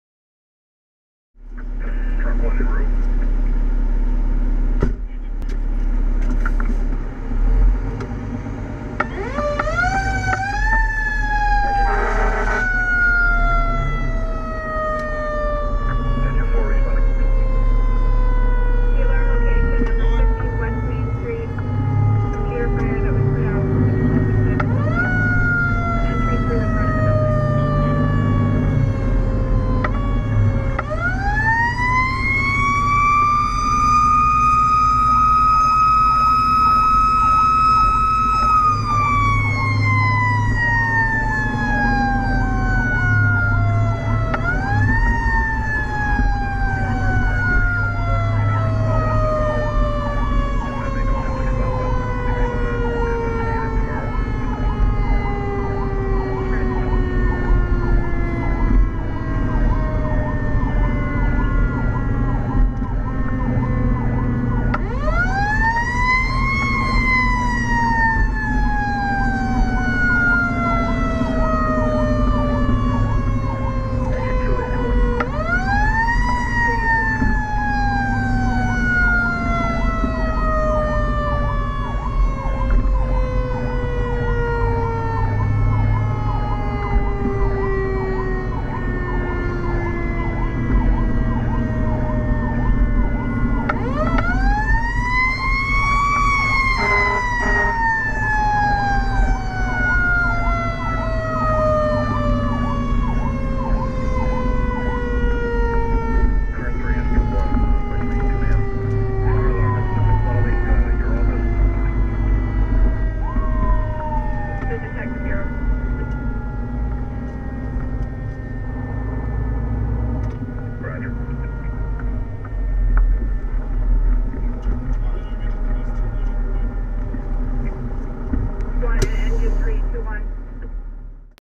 GoPro Series | Engine 3 Responding to 50 West Main Street
50 West Main Street for a computer on fire that was extinguished upon companies arrival. GoPro Hero 3+ mounted on driver's side, inside.